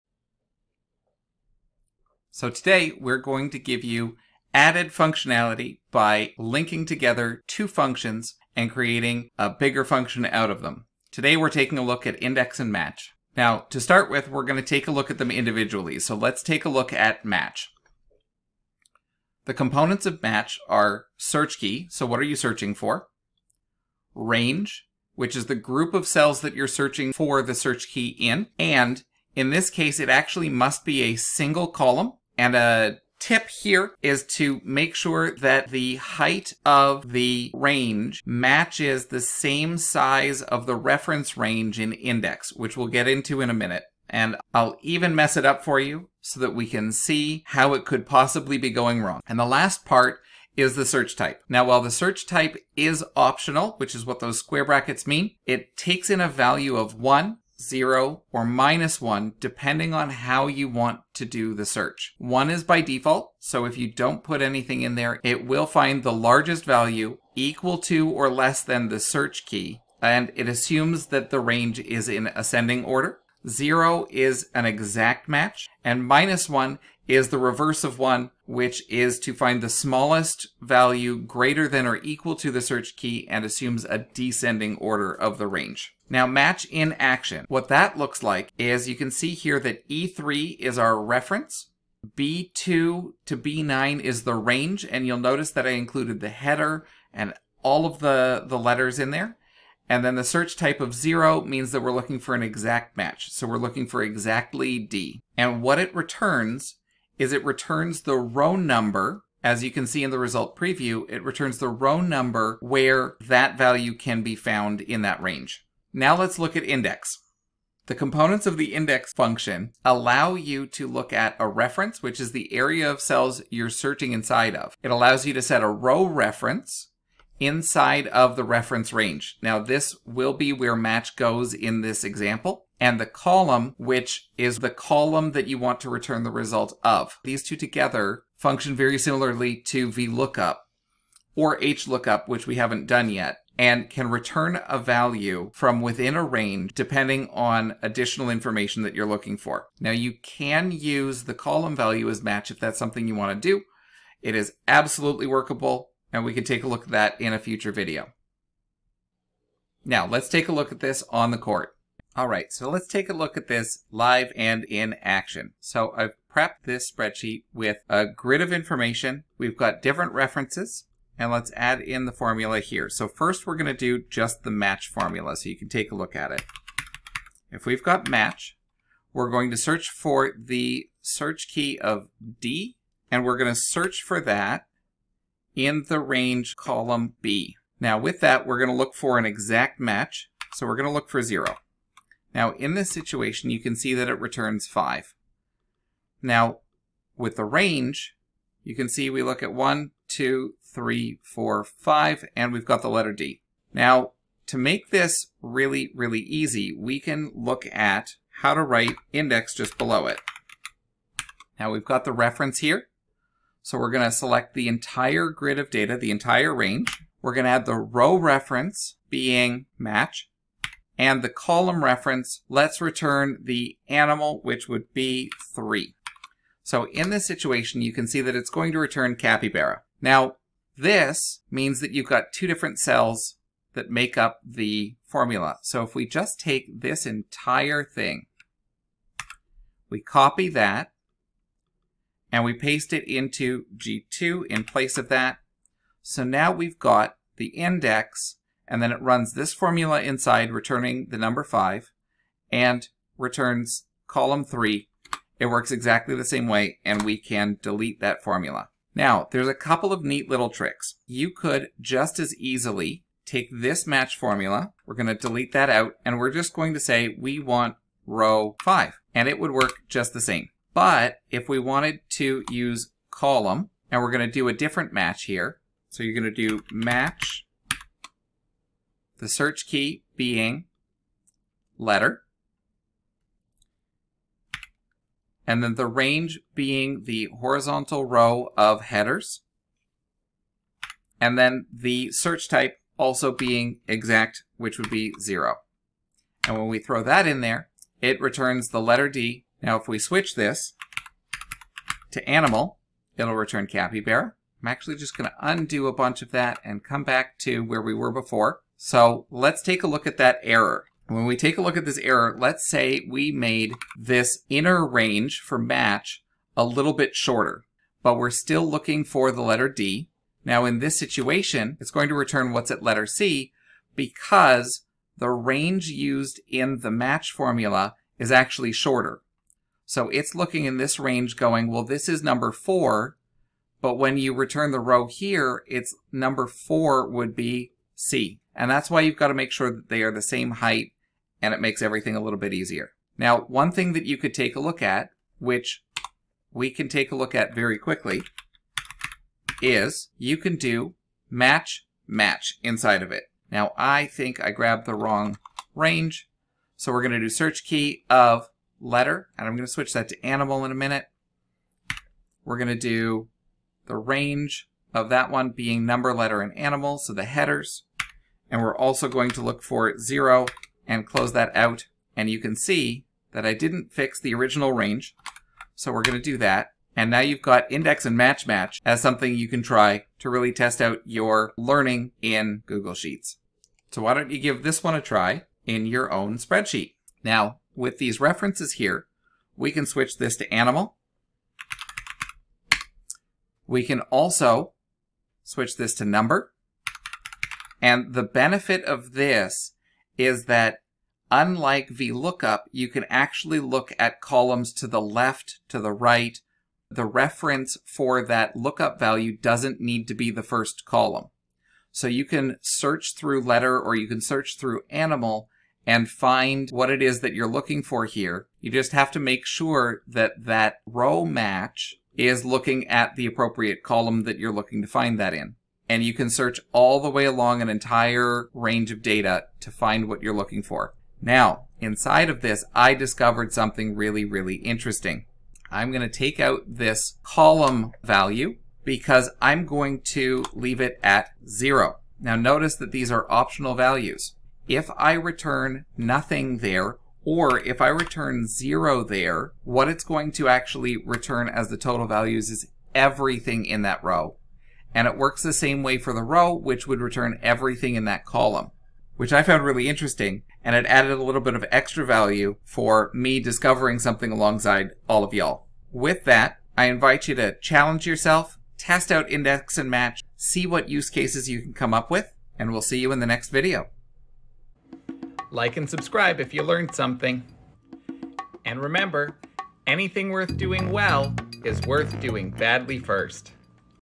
Google Sheets: Use Index and Match Together for Advanced Lookups
A few weeks ago we did a video on VLOOKUP. Now we are taking it to a new level with INDEX and MATCH. 🔥

While they may look like they do the same, INDEX and MATCH together brings a whole new dimension to your lookups allowing you to return entire rows or columns, or to return values to the left of the search key. This is a game changer when building a great spreadsheet.

Learn how to combine formulas and create even greater functionality with these two essential formulas.

00:00 Introduction
00:17 MATCH components
01:56 INDEX components
02:42 Tutorial in the spreadsheet
05:11 Troubleshooting errors
05:45 INDEX MATCH MATCH
07:20 Return complete rows/columns